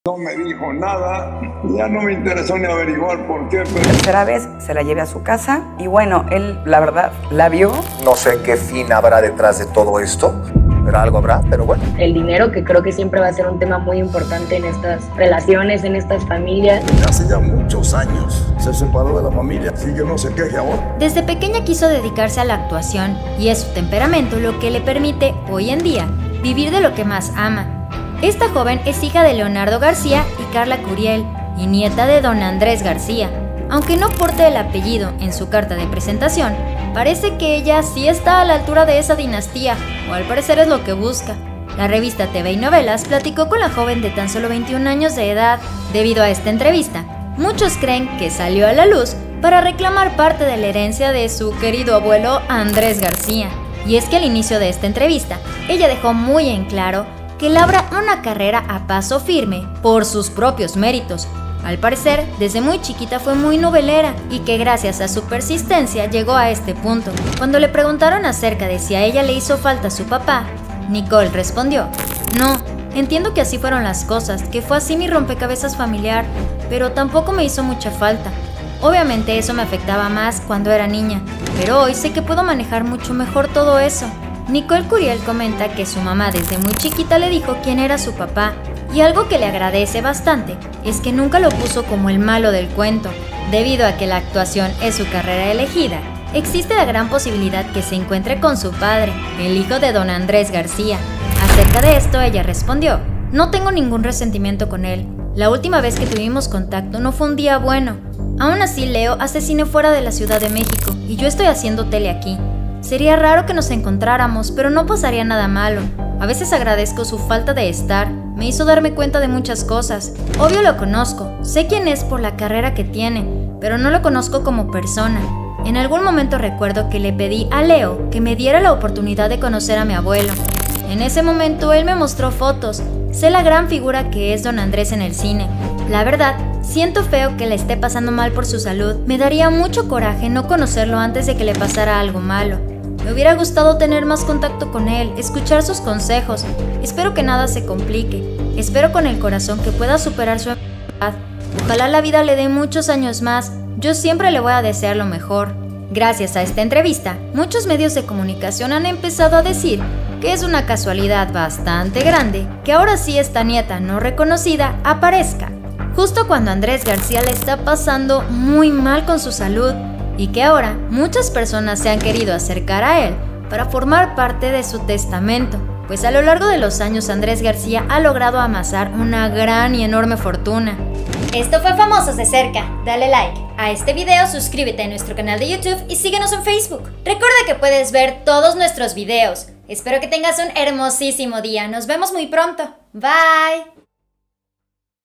🔥🔥𝗡𝗶𝗲𝘁𝗮 𝗡𝗼 𝗥𝗲𝗰𝗼𝗻𝗼𝗰𝗶𝗱𝗮 𝗱𝗲 𝗔𝗻𝗱𝗿é𝘀 𝗚𝗮𝗿𝗰í𝗮 𝗘𝘅𝗶𝗴𝗲 𝗘𝗻𝘁𝗿𝗮𝗿 𝗲𝗻 𝗲𝗹 𝗧𝗲𝘀𝘁𝗮𝗺𝗲𝗻𝘁𝗼 𝘆 𝗥𝗲𝗰𝗶𝗯𝗶𝗿 𝗣𝗮𝗿𝘁𝗲 𝗱𝗲 𝗦𝘂 𝗙𝗼𝗿𝘁𝘂𝗻𝗮😱
Nicole Curiel una joven de 21 años asegura que es hija de Leonardo Garcia y que su abuelo es Andrés Garcia, la joven tambien afirmo que le urge encontarse con el actor pues no quiere que el histrion parta de este mundo sin llegar a conocerlo personalmente.

Bitcoin:
Trading:
ecommerce: